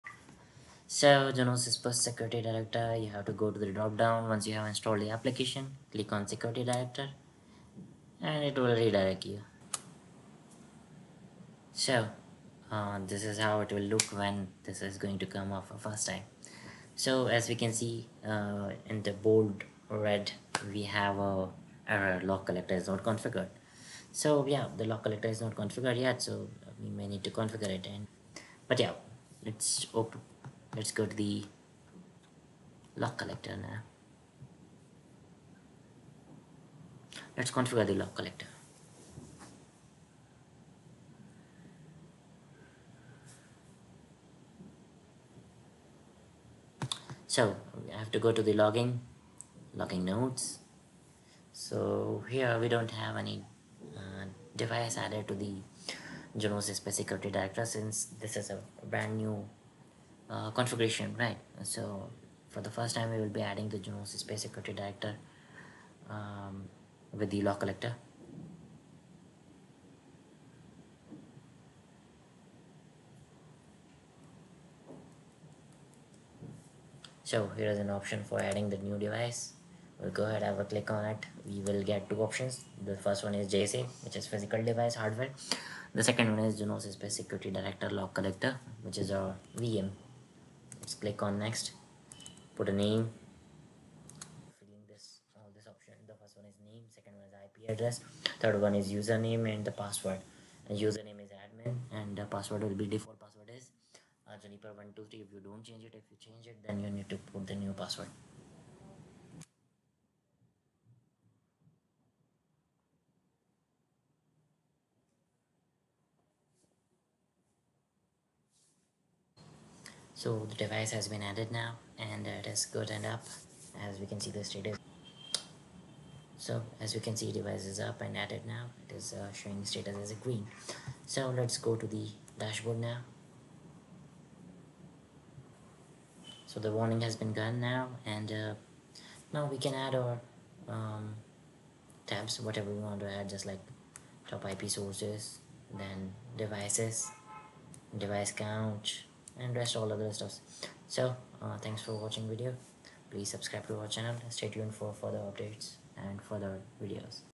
Add a Log Collector in Junos Space. Configure Log collector in Juniper security director
Login to device for first time and Configure Junos Space log collector. Also customize your Dashboard.